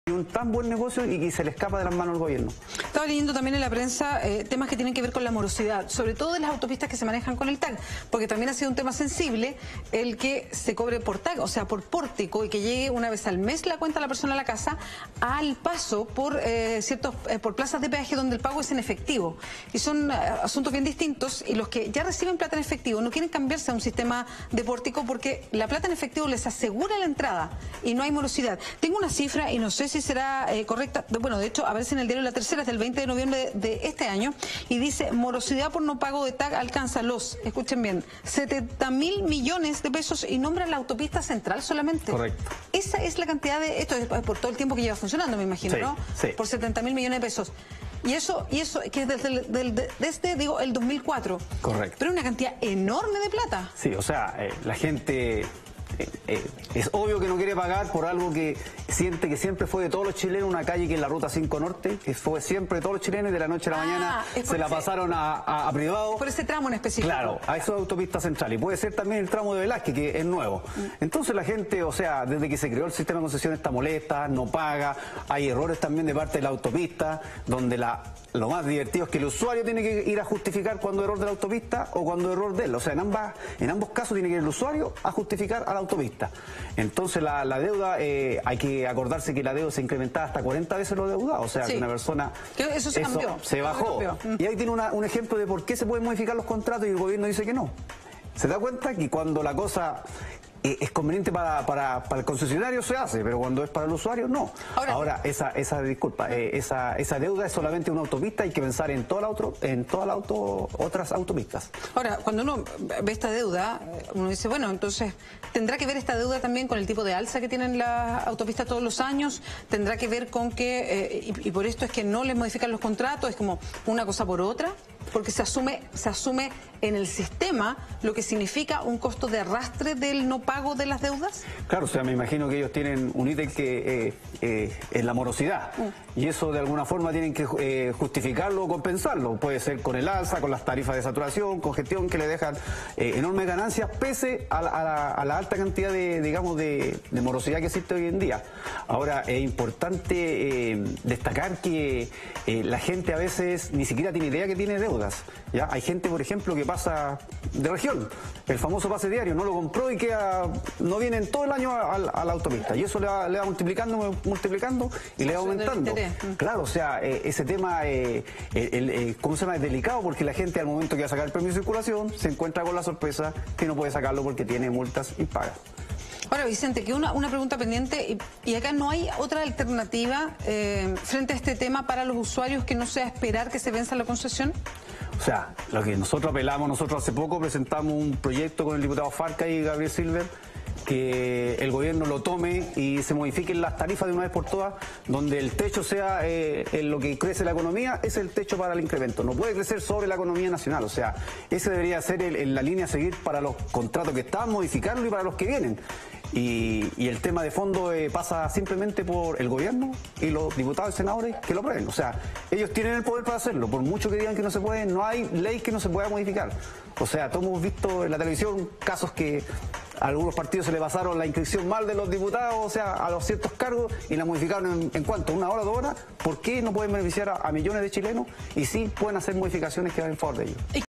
La gran cantidad de automovilistas morosos en las autopistas
Vicente Faundes, presidente de Automovilistas Unidos de Chile, comentó en nuestro programa las razones de por qué la gente no paga las deudas en el TAG.